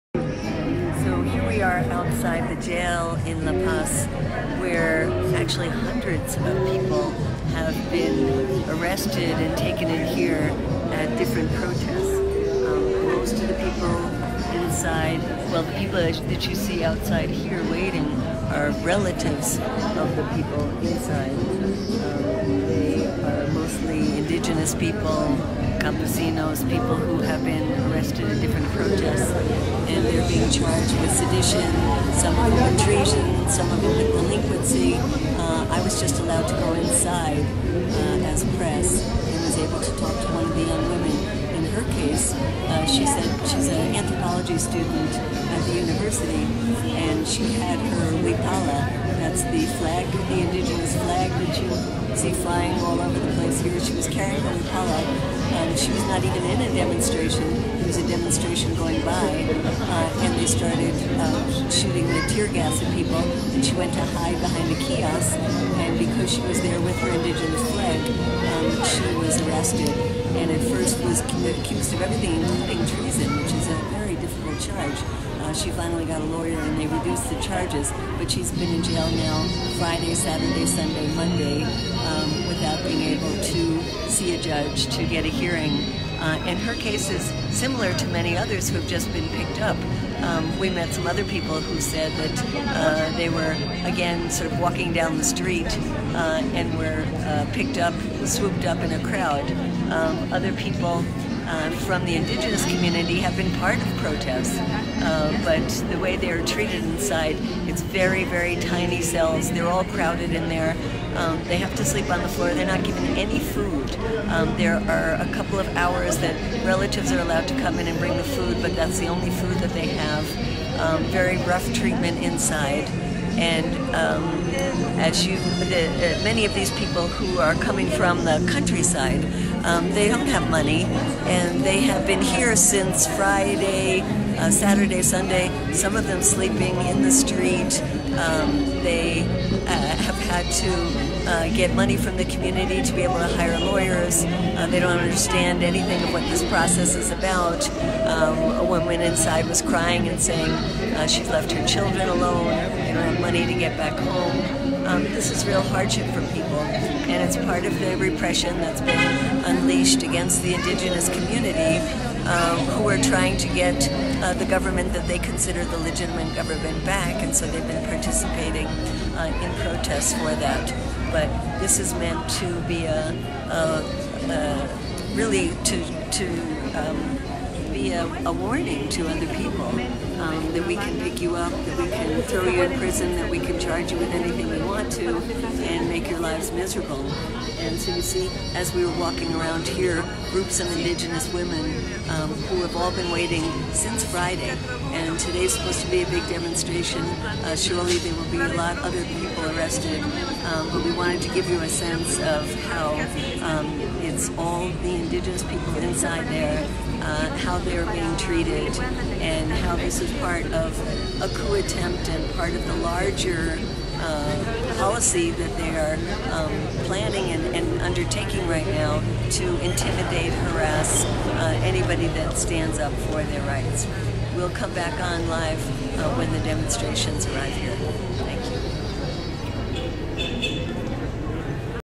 Bolivia: Protesters and bystanders arrested without legal hearing
Medea Benjamin reports from outside a jail in La Paz, Bolivia, where the friends and family of detained protesters wait for their release.

Medea explains how peaceful Bolivian protesters, even bystanders, have been arrested for days without a hearing.